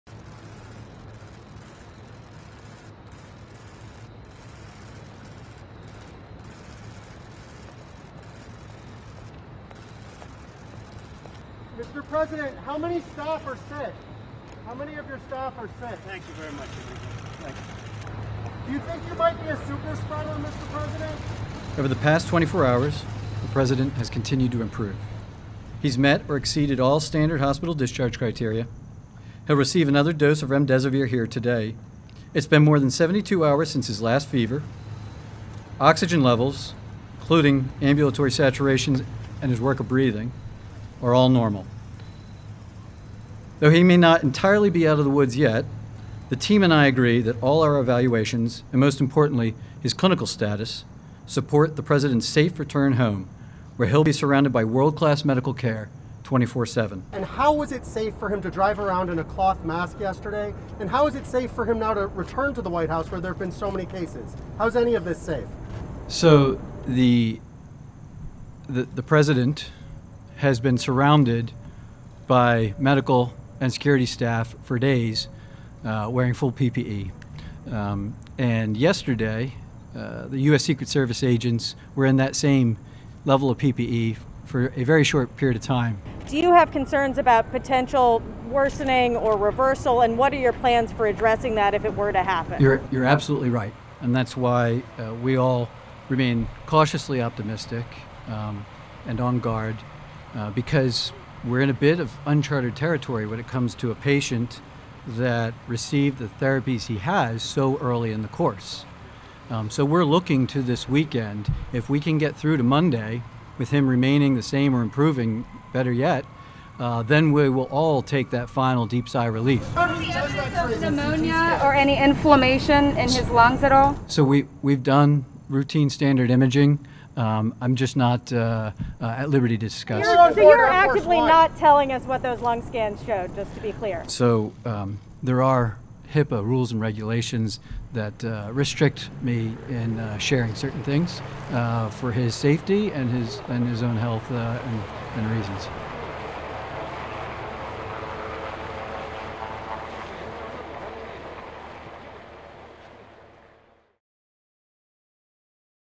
Trump leaves hospital after coronavirus treament but ‘not out of woods’
Donald Trump has left Walter Reed National Military Medical Center after spending several days receiving treatment for Covid-19. Trump’s doctors said the US president was ‘not yet out of the woods’ but had met all standard hospital criteria to be discharged. Trump’s physician, Dr Sean Conley, said: ‘We remain cautiously optimistic and on guard because we're in a bit of uncharted territory when it comes to a patient that received the therapies he has so early in the course.' When asked by reporters Conley said he was 'not at liberty to discuss' Trump’s latest lung scans due to health privacy regulations.